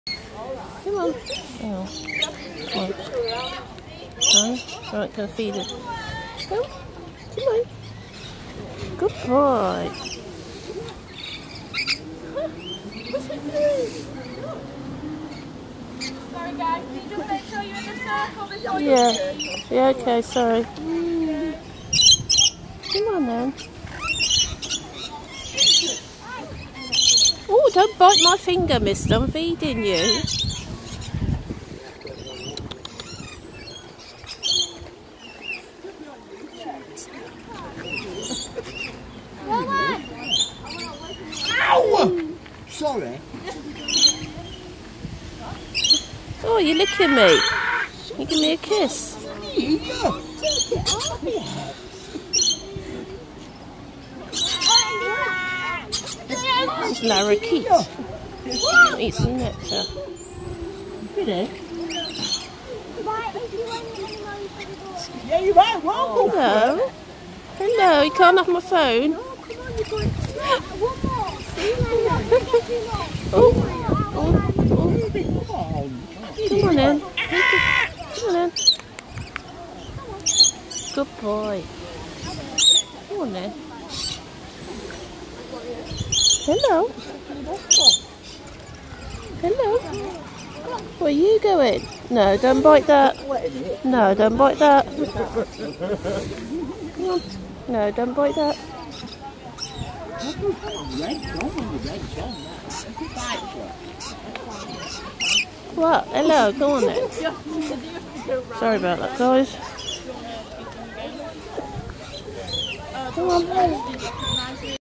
Blackpool zoo
Feeding a lorikeet bird n another lorikeet bird decided to, land on my phone